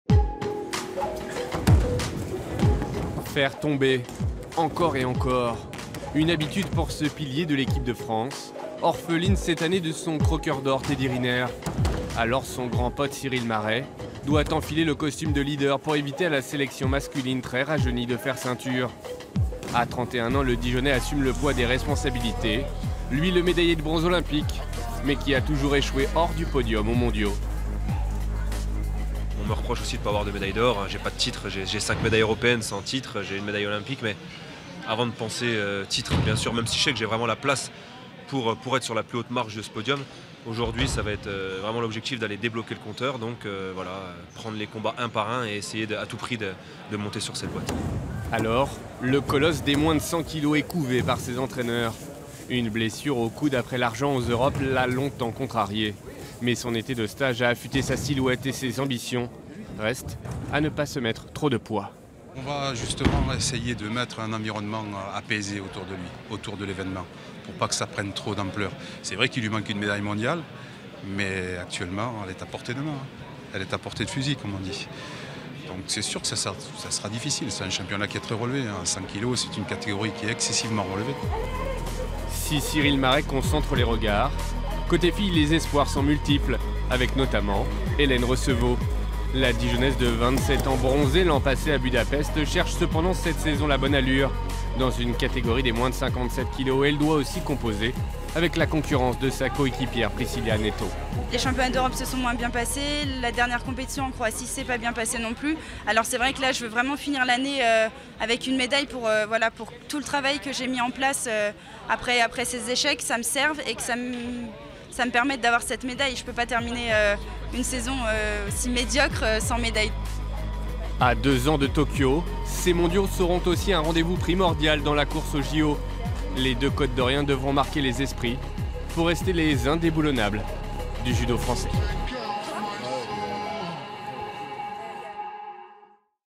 Deux judokas bourguignons engagés aux championnats du monde
Deux Bourguignons aux championnats du monde de judo à Bakou en Azerbaidjan. Cyrille Maret et Hélène Receveaux, sérieux prétendants à un titre.

Christophe Tarrisse et Romain Liboz les ont suivis à l'INSEP à Paris dans leur entrainement quelques jours avant la compétition.

Reportage avec :
Cyrille Maret, médaillé de bronze olympique en 2016
Richard Melillo, entraîneur de Cyrille Maret
Hélène Receveaux, médaillée de bronze mondiaux 2017